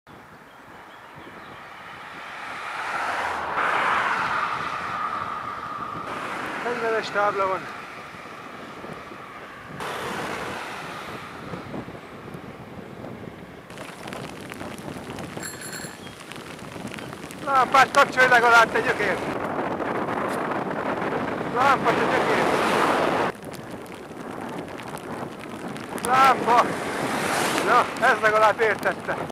20230608 Road monkeys
No lights even in rain.
Úti majmok, még esőben sem használnak világítást.